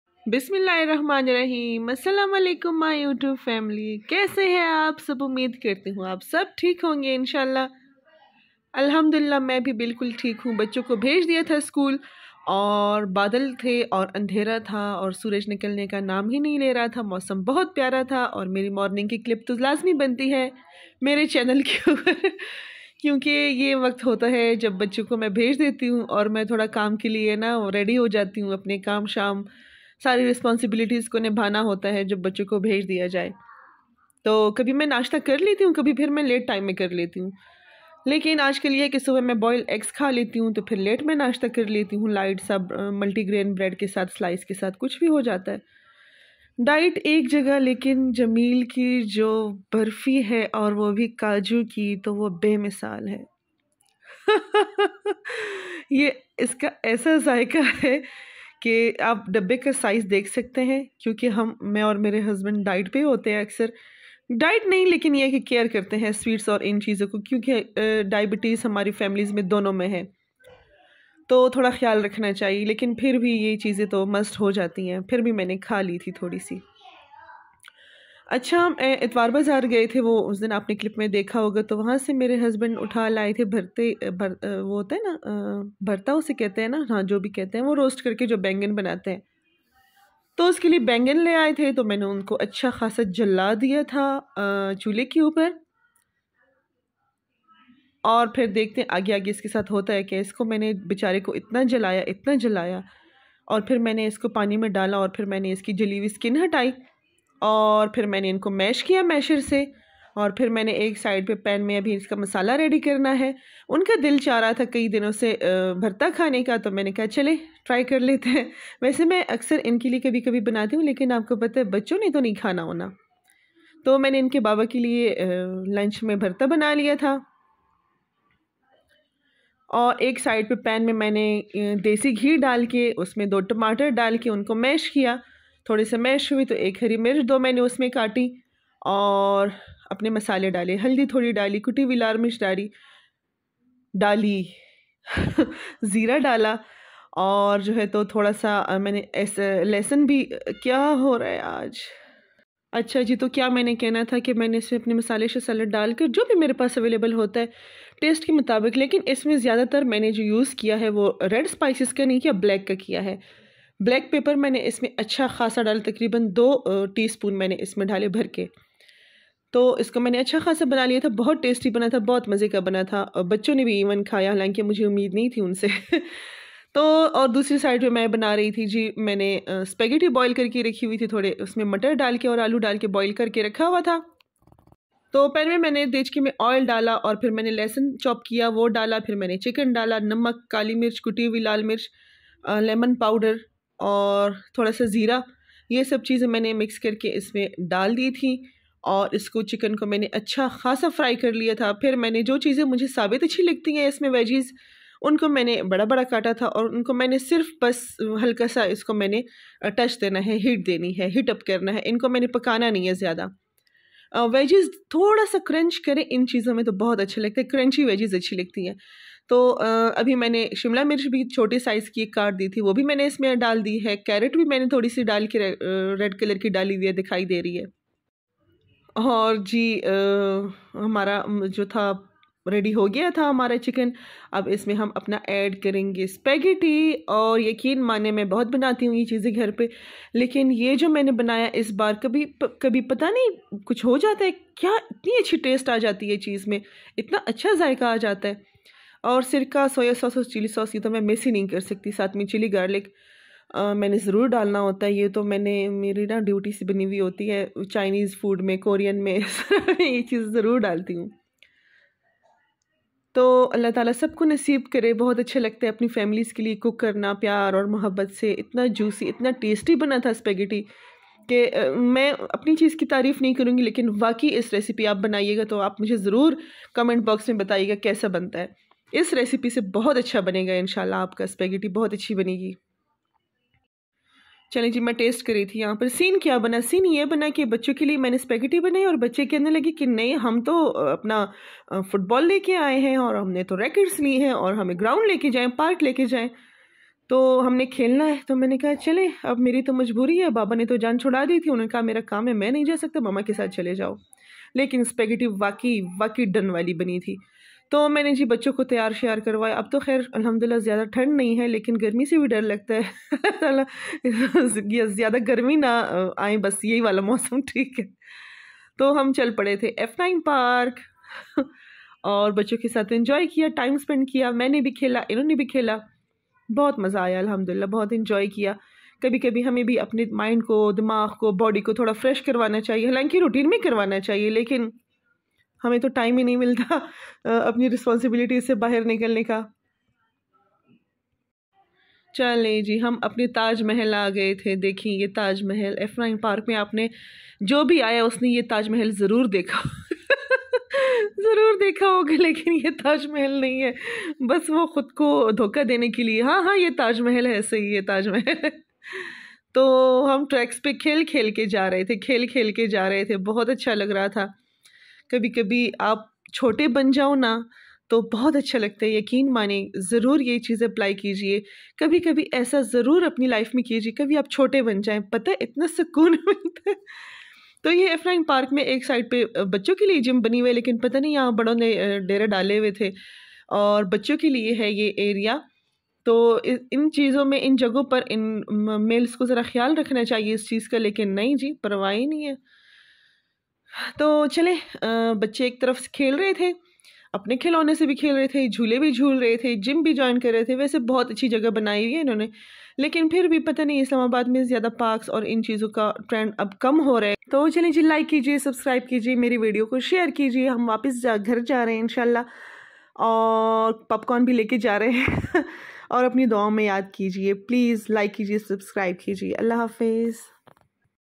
India ka taj mahal | Taj mahal in Islamabad | tasty juicy speghtti | kitchen Queen #
Taj mahal in Islamabad |  juicy tasty speghtti recipy | bachon k sath kbhi bacha bna k dekho kitna acha feel hota hai #